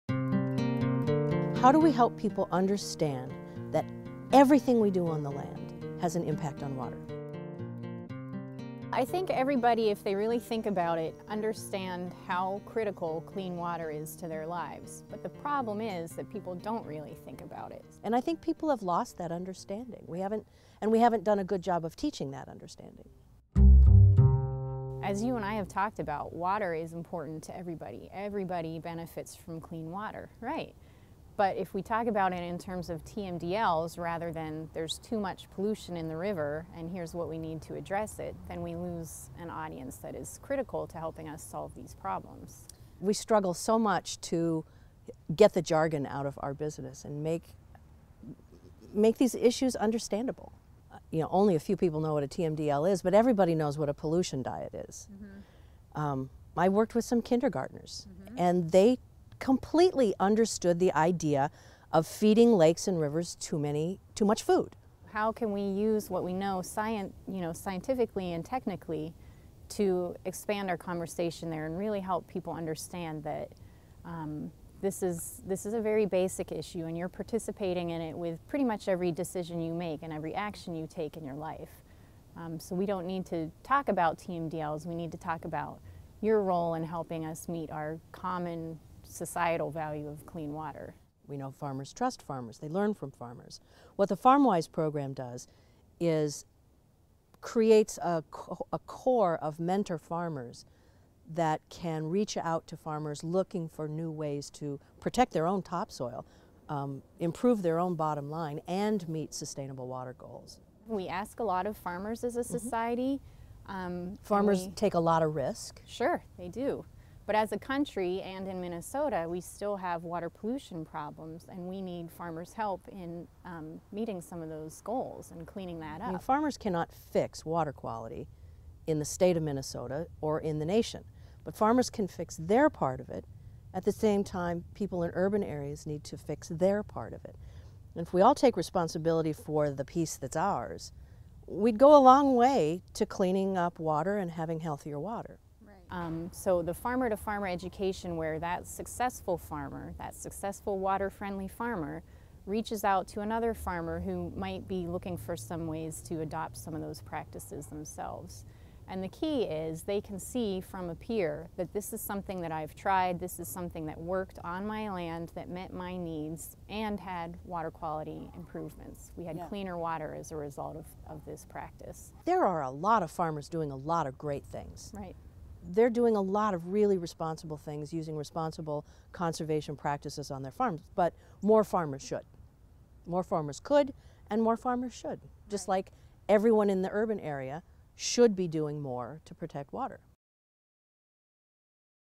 Kitchen Table Conversations: FarmWise for Clean Water
Tackling an issue like agricultural runoff and water quality is a long term goal, and it's difficult to pinpoint the role for the public sector.  Lark Weller and Peggy Knap founded Minnesota FarmWise, a farmer-farmer mentorship program aimed at improving water quality practices state-wide, starting at local levels.
They hope to improve water quality literacy in Minnesota farmers, and teach us all that all our decisions have some impact on water quality for the future.